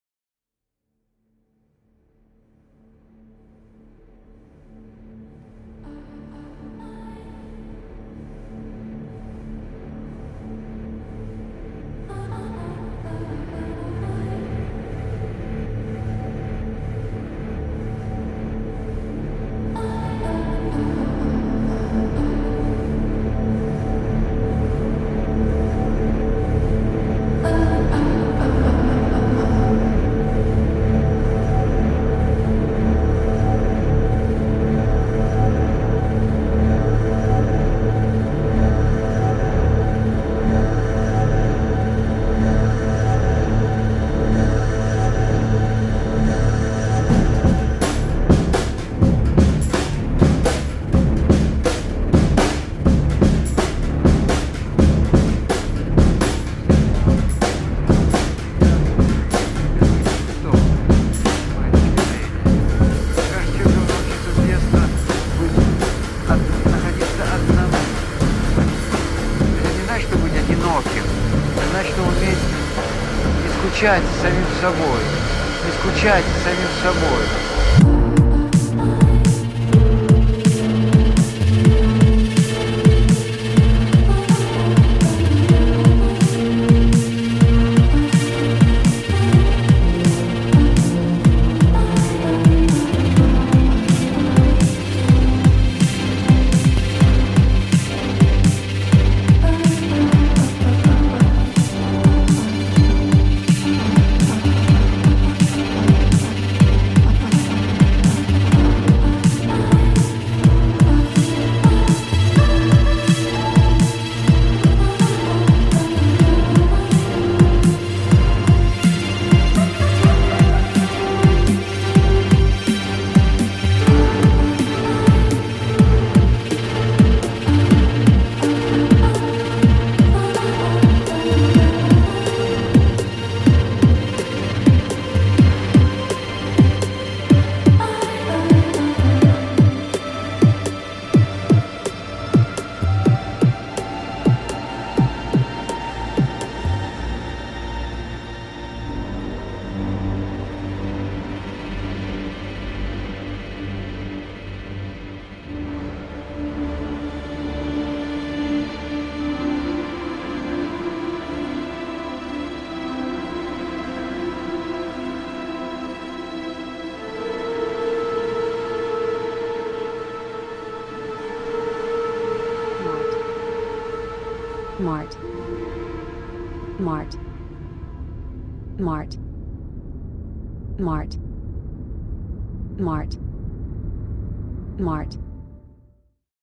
MART выставка // 18 мая //
Выставка MART ― это сеанс психотерапии, коридор смирения со своими демонами.
Приходи 18 мая к MART и Ты увидишь здесь себя.
Вход свободный. Как ты.